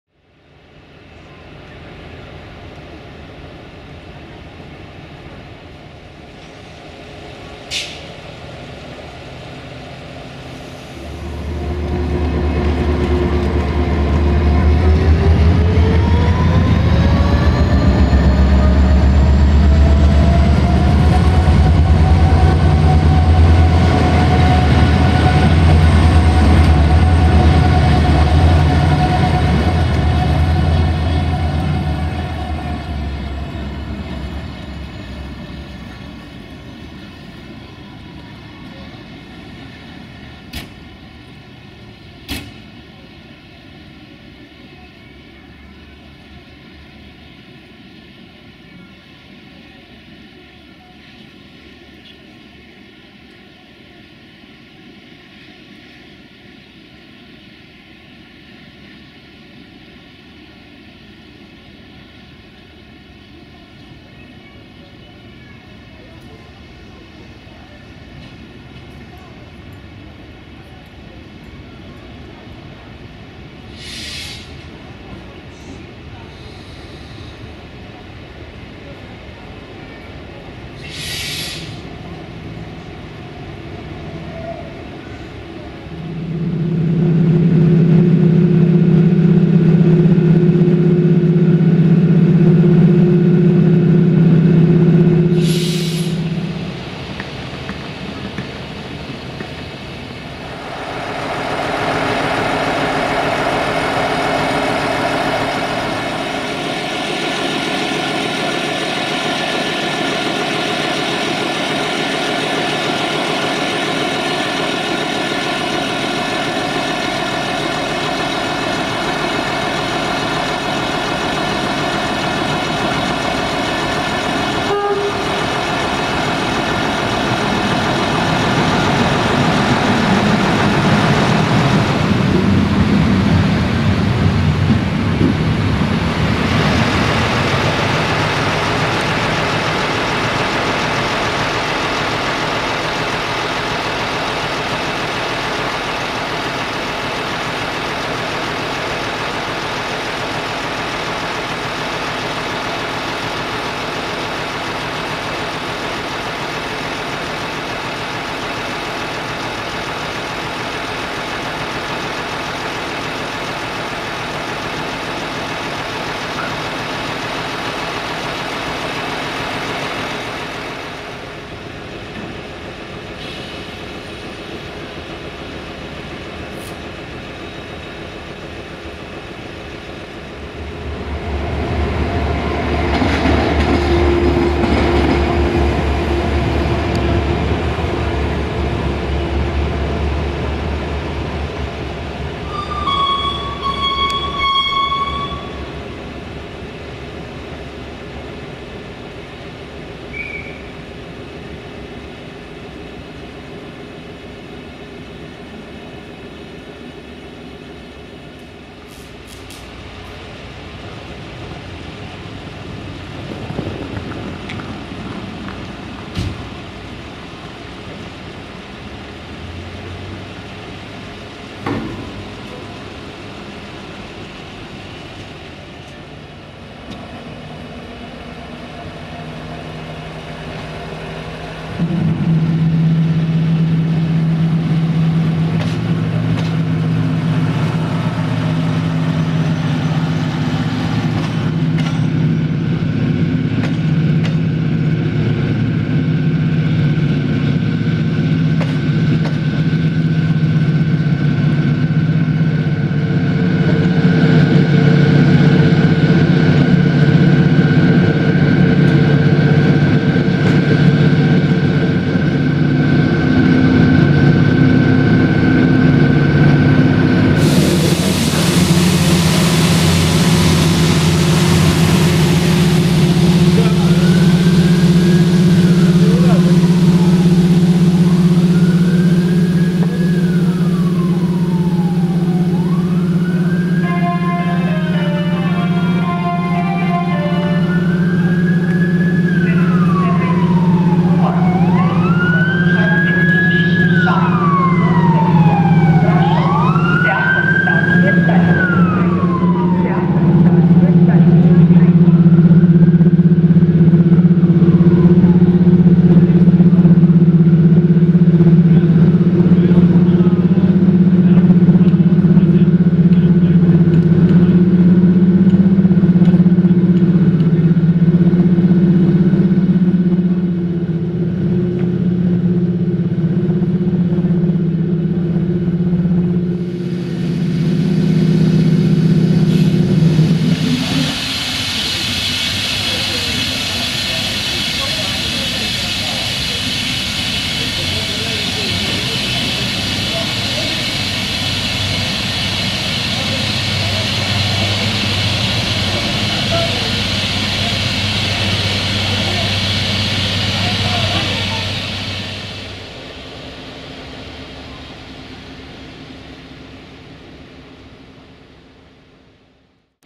Activitate Feroviară în Gara Cluj Napoca/Rail Activity in Cluj Napoca Station - 05 February 2021
00:12 Manevră/Shunting - Jimmy Tricolor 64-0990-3 RO-SNTFC
00:40 Manevră/Shunting - Automotor Duewag BR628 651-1 DMU IRC/DB Regio
01:46  Manevră/Shunting - Clujeanca 060-EA/LE5100KW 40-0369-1 RO-SNTFC
03:02 Manevră/Shunting - Jimmy Tricolor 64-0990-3 RO-SNTFC
03:47 Manevră/Shunting - Automotor Duewag BR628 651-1 DMU IRC/DB Regio
04:25 Manevră/Shunting - Automotor Săgeată Albastră Desiro VT642 DMU RO-SNTFC
05:28 Manevră/Shunting - Automotor Săgeată Albastră Desiro VT642 DMU RO-SNTFC

05 February 2021